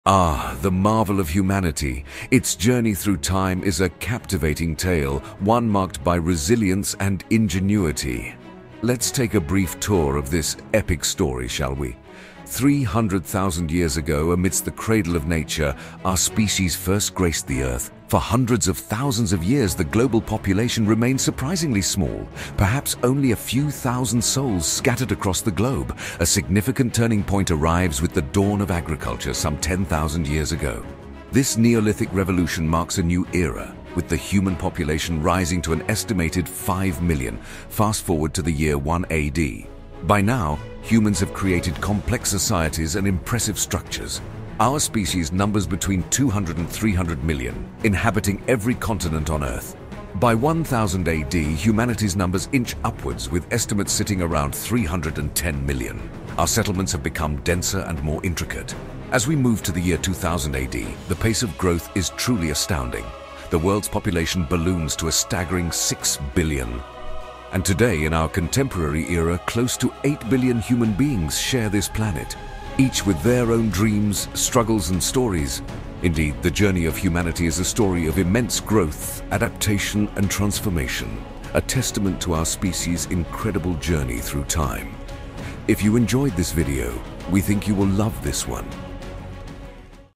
From Dawn to Billions: The History of Human World Population
Join us on an enthralling journey through time as we trace the remarkable growth of humanity, from our earliest beginnings to the contemporary era. In a span of 300,000 years, our species has gone from a humble population of a few thousand to an astonishing 8 billion. Uncover the significant milestones and revolutions that have shaped our history and our world. This short film marries the best of nature narration with intriguing scientific insights. So, step into the time machine and let's embark on this captivating journey together.

Enjoy the tunes in our videos? Why not add some to your own content with this